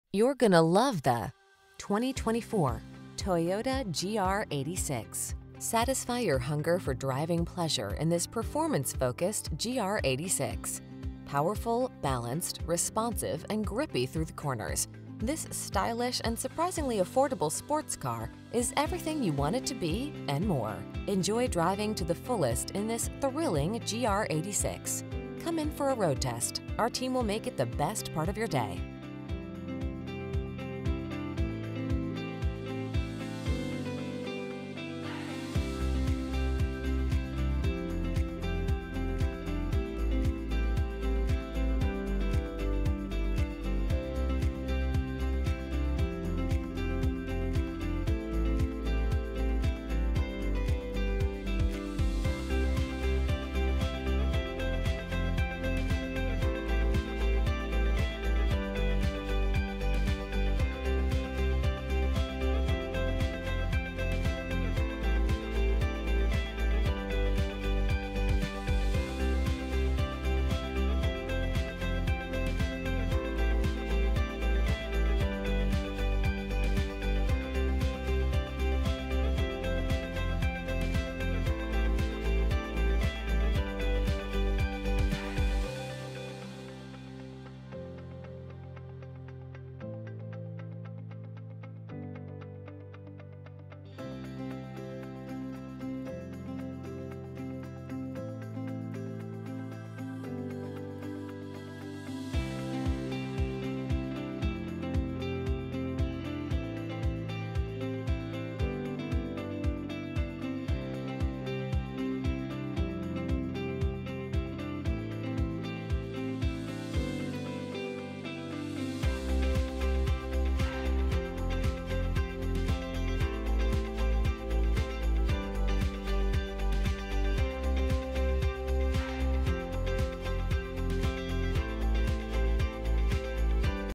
2024 Toyota GR86 Northern California, Redding, Sacramento, Red Bluff, Chico, CA R9751512
New 2024 Toyota GR86  available in Northern California at  Lithia Toyota of Redding. Servicing the Redding, Sacramento, Red Bluff, Chico, CA area.

250 E. Cypress Ave.